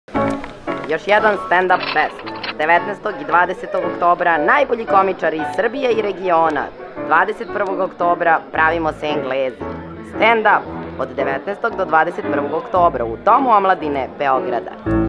StandUp Fest 2012
StandUpFest2012

19, 20. i 21. oktobar u Domu Omladine Beograda -- 20 komičara iz celog sveta
organizacija: Standup.rs
Međunarodni festival StandUpFest2012 okupiće najbolje stand up komičare iz Srbije i regiona, kao i komičare iz Velike Britanije i Kanade -- ukupno 20 izvođača na velikoj sceni Doma omladine Beograda.

Illustration by Dobrosav Bob Živković
Animation by Nikola Silić